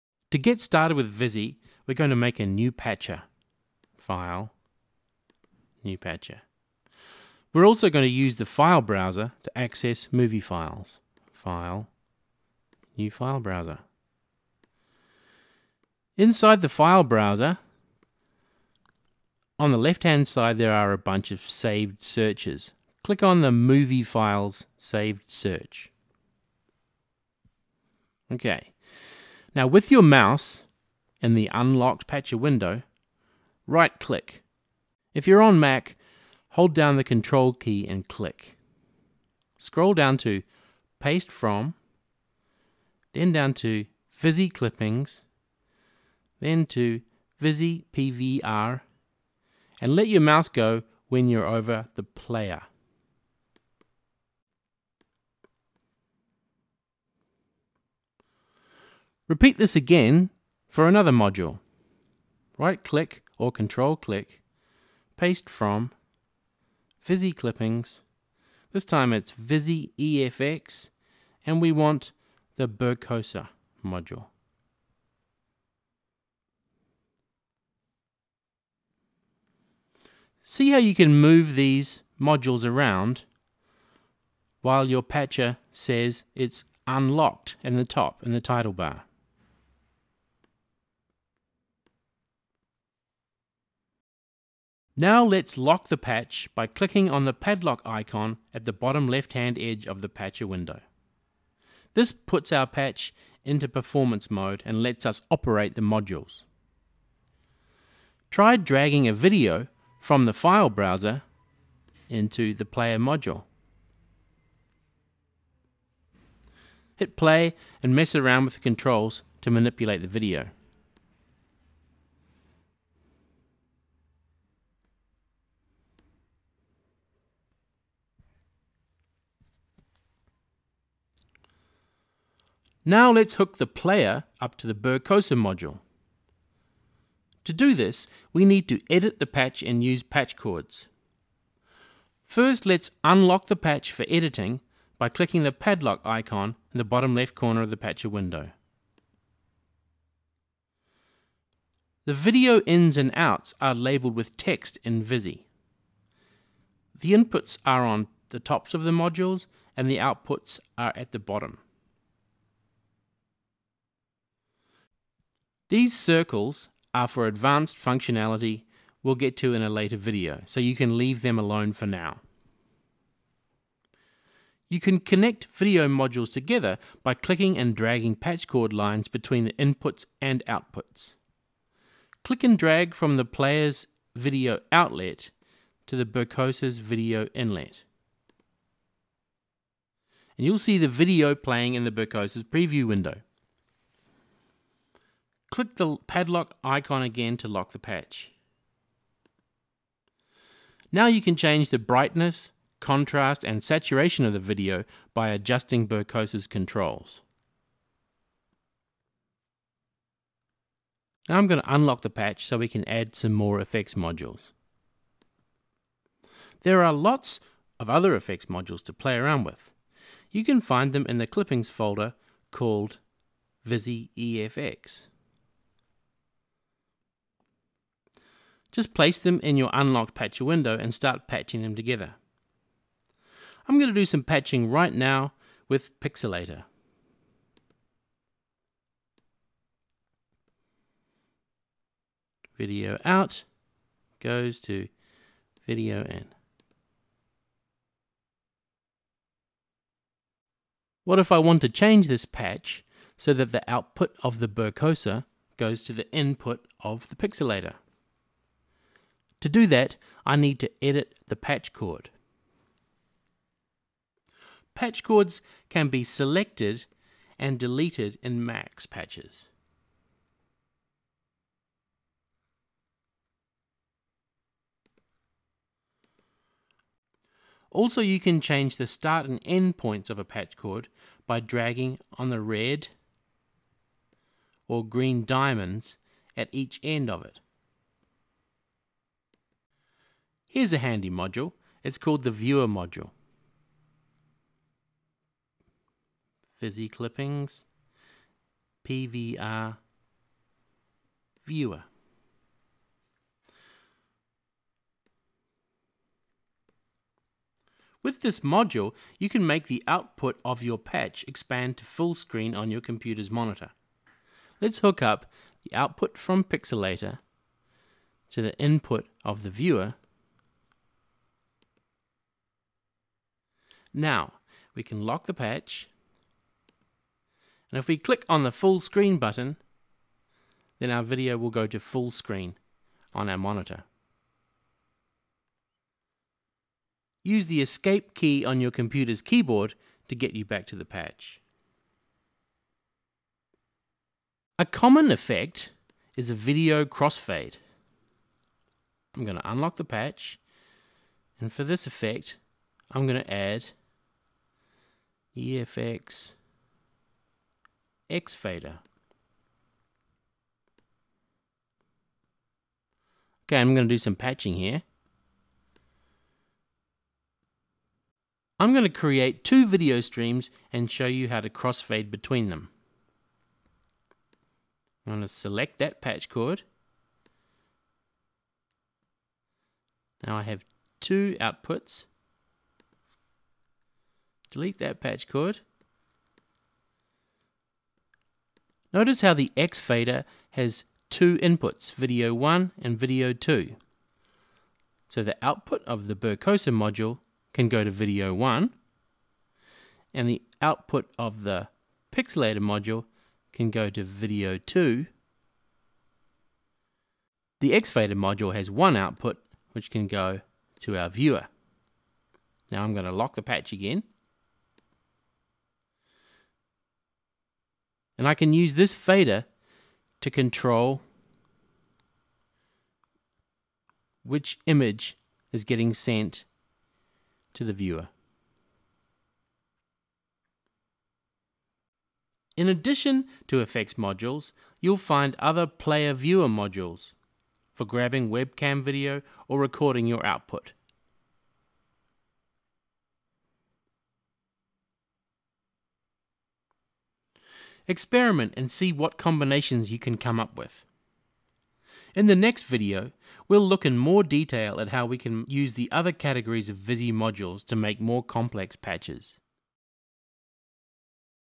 Cycling74 Vizzie Tutorials: Quickstart - 1
Get started using VIZZIE, the quickest and easiest way to build Jitter patches.